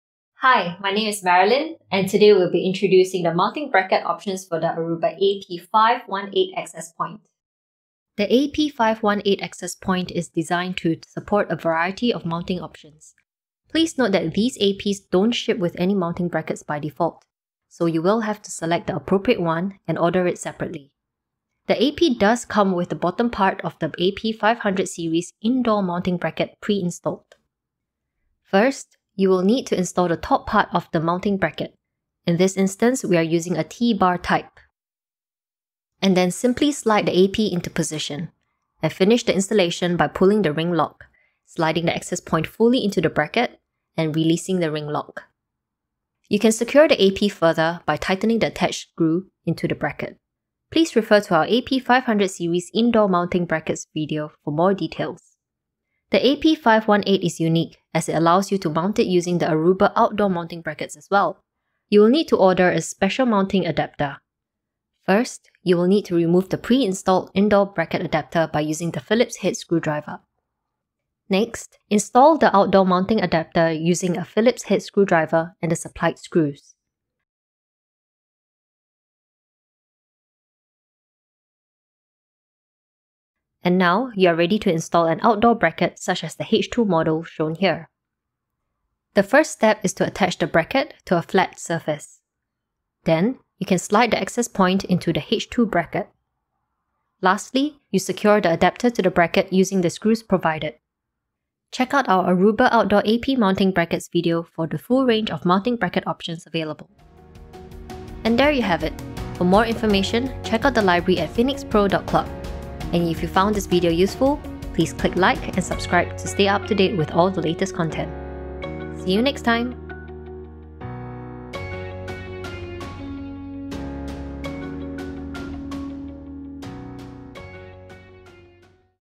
Aruba AP-518 Mounting Bracket Guide
Learn about the different mounting bracket options for the Aruba AP-518 ruggedised indoor access points | More